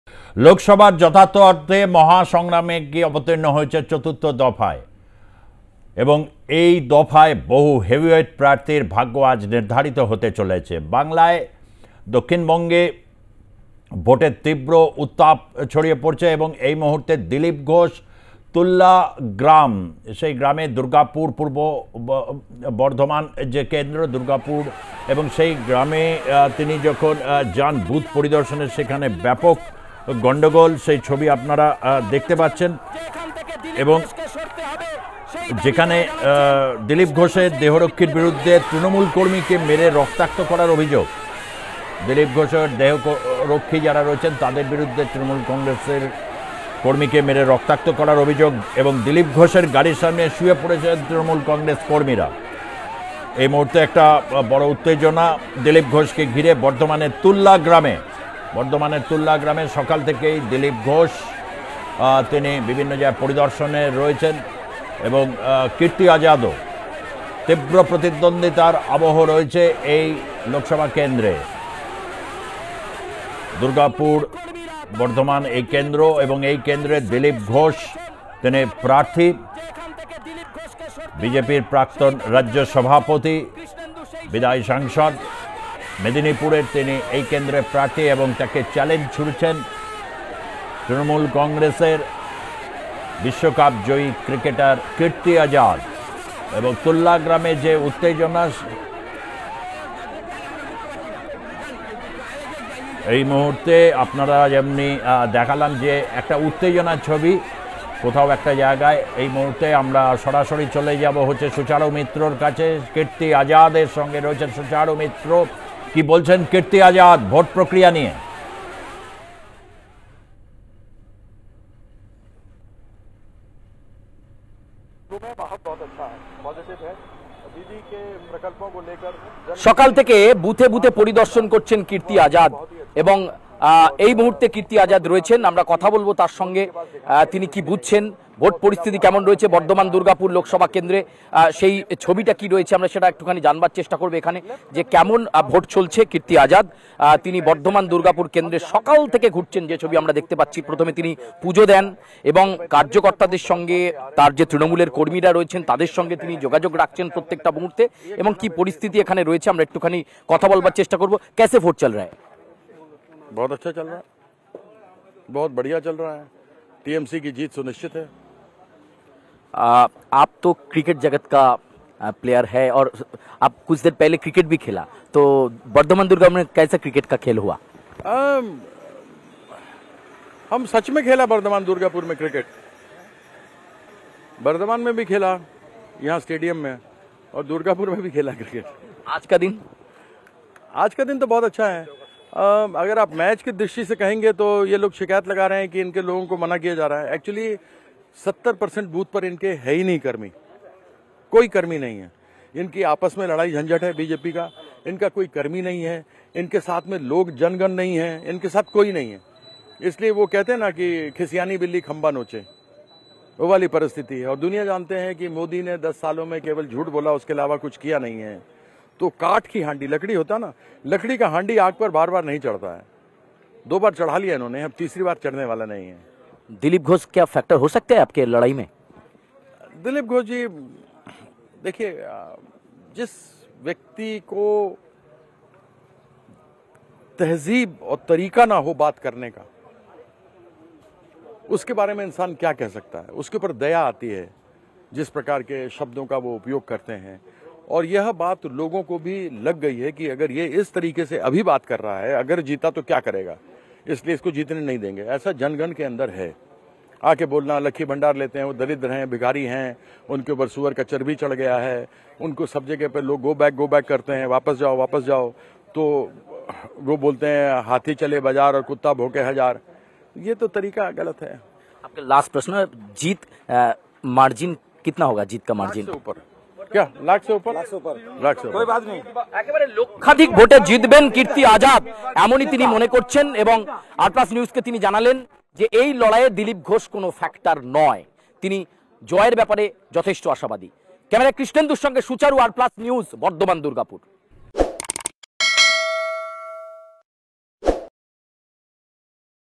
Kriti Azad, Lok Sabha Election 2024 : দিলীপ ঘোষ কোন ফ্যাক্টর নয়
Kriti Azad, Lok Sabha Election 2024 : দিলীপ ঘোষ কোন ফ্যাক্টর নয় | তুল্লা গ্রামে বুথ পরিদর্শনে যাওয়ার সময় ব্যাপক গণ্ডগোল | R PLUS NEWS